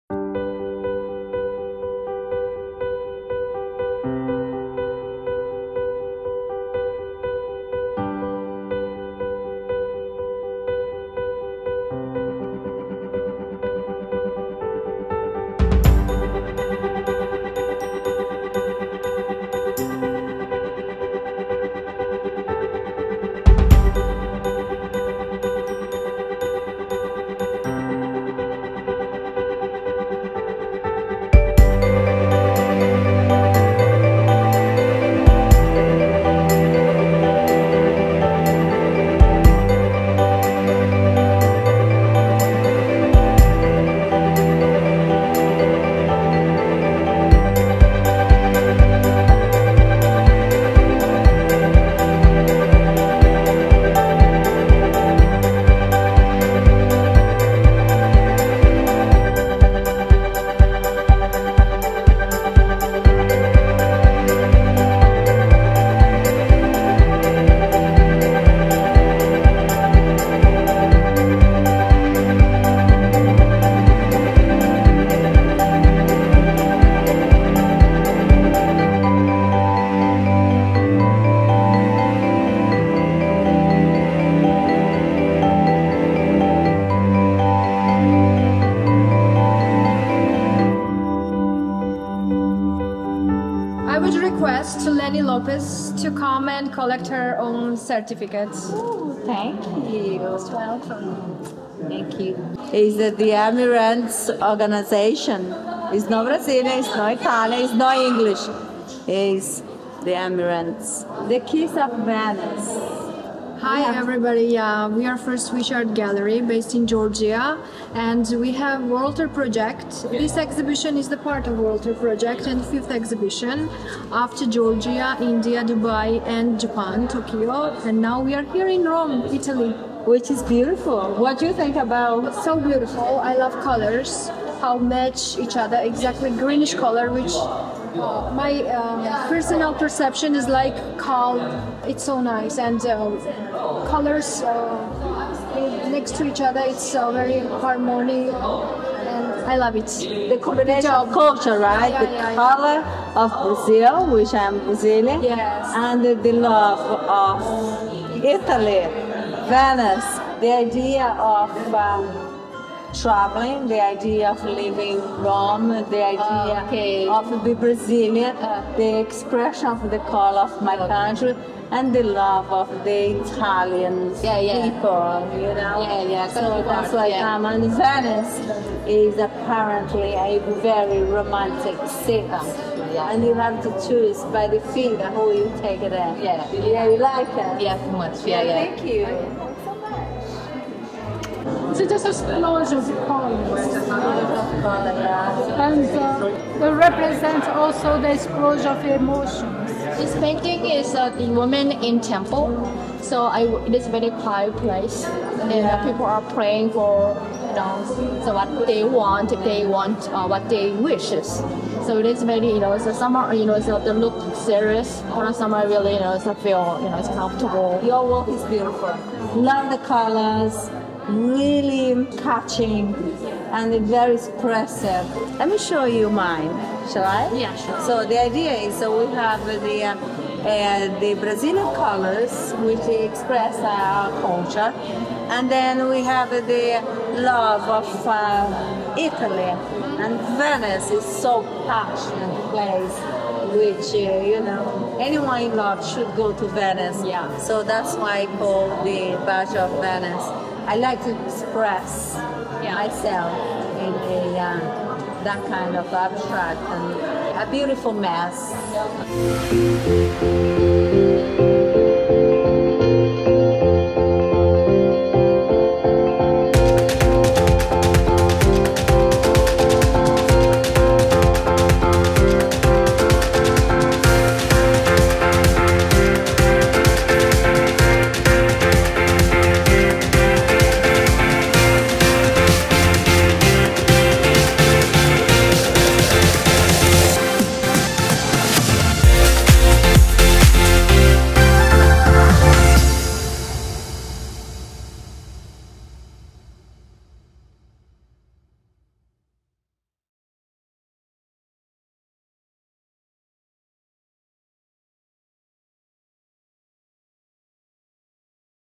LENNY LOPES. Mission World Tour Exhibitions - Rome, Italy:
Mission World Tour Exhibitions - Rome, Italy:
Unveiling Creativity and Culture
Venue: Museum Venanzo Crocetti, Rome - Italy Address: Via Cassia, 492, 00189 Roma
Exhibition Schedule:
Artwork Arrival: September 8th and 9th, 2023
Experience the anticipation as masterpieces from around the world find their home within the hallowed halls of the Museum Venanzo Crocetti.
Exhibition Setup: September 11th, 2023
Witness the transformation of blank spaces into captivating galleries, as artistic vision takes form under the skillful hands of our team.
Vernissage: September 12th, 2023 @ 17:00
Step into a world of wonder and creativity as we celebrate the grand opening of the Mission World Tour Exhibition. Join us for a captivating afternoon where art, culture, and imagination converge.
Curator and Expert Presentation:
Prepare to be guided through the artistic tapestry by the expertise of acclaimed art curator and critic Giorgio Bertozzi, and the insightful perspective of Claudio Giulianelli. Their passion and insights promise to deepen your connection with each masterpiece.